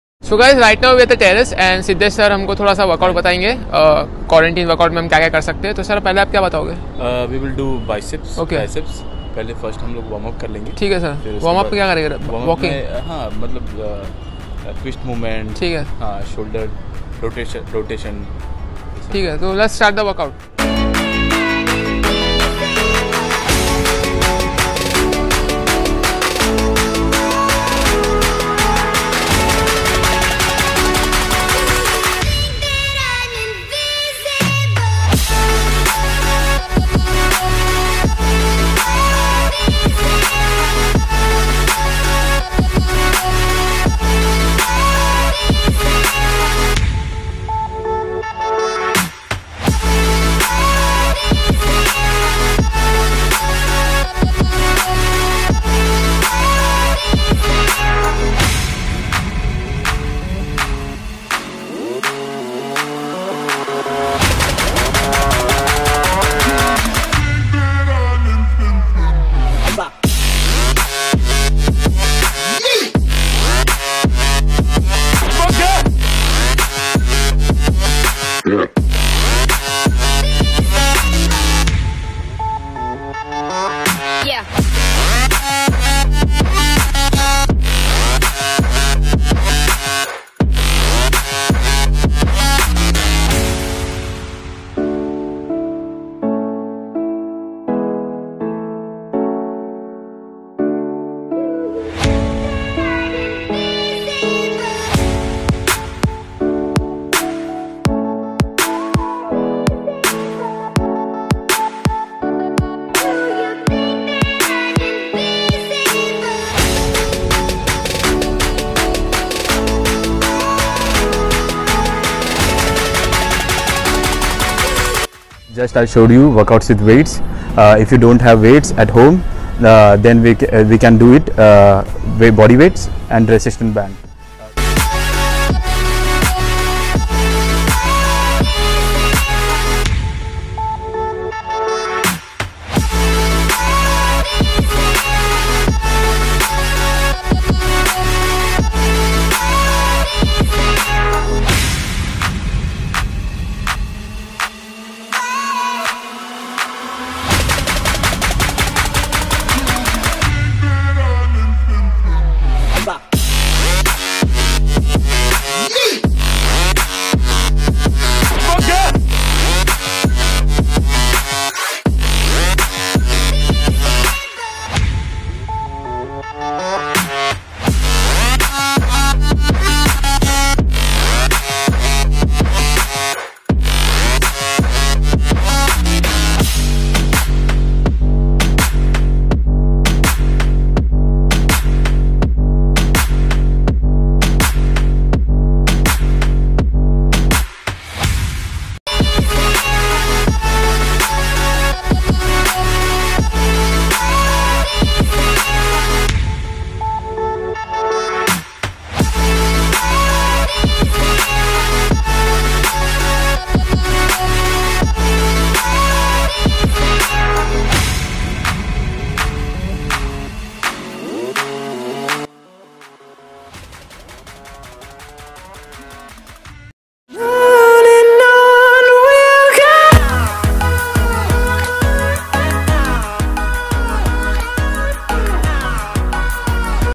The Best Full Body Home workout with Mr Siddhesh Golds Gym Personal Trainer
In part two of our Interview series with Mr Siddhesh we bring you full body home workout which include bodyweight, resistance band & Dumbbell workouts. These home full body workout is great for fitness and keeping healthy during lockdown as you can easily workout at home.

Do watch this video and try these easy to do home exercises to stay fit & healthy during the lockdown